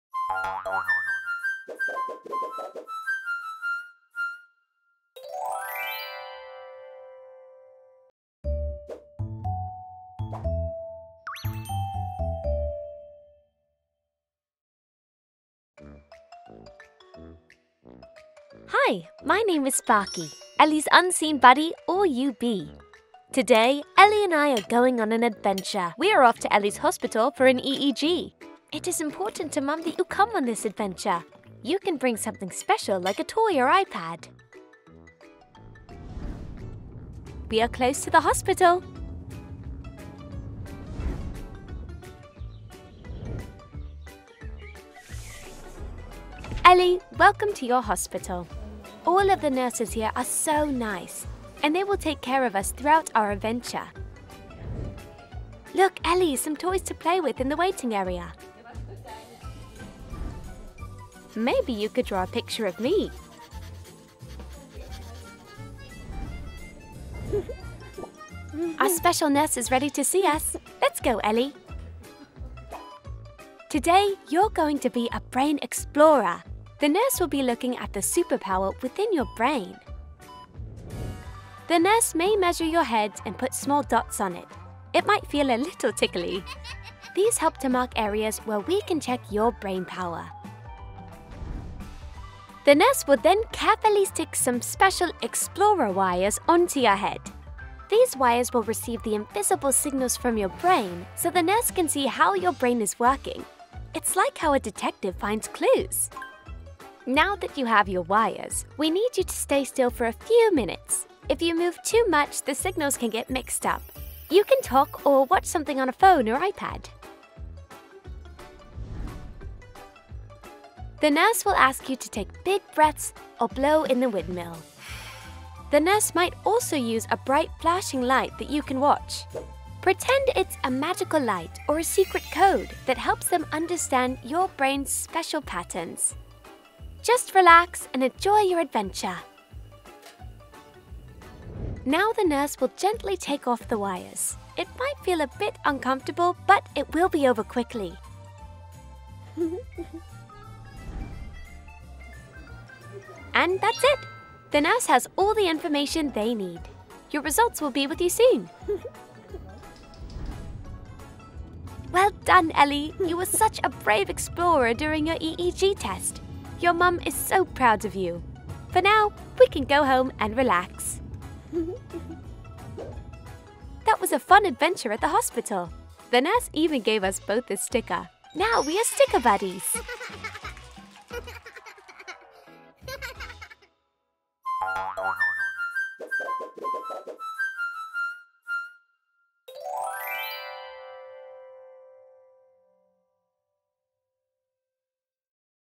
Going for an EEG: What to Expect – A Friendly Guide for Kids & Families
Is your child going for an EEG?
This gentle, animated guide is designed to help children (and their families) understand what to expect during an EEG (Electroencephalogram) appointment.

Created especially for children with additional or special needs, this video explains the process in a calm, friendly, and visual way – helping to ease anxiety, answer common questions, and support caregivers.

🎥 What’s inside:

✅What an EEG is
✅Why it's done
✅What happens before, during, and after
✅Friendly characters and clear, comforting narration
✅Perfect for children who are visual learners

🧠 Whether your child is feeling nervous or just curious, this video helps make the experience of going for an EEG less scary—and more empowering.

📌 Support your child’s journey with knowledge, reassurance, and calm.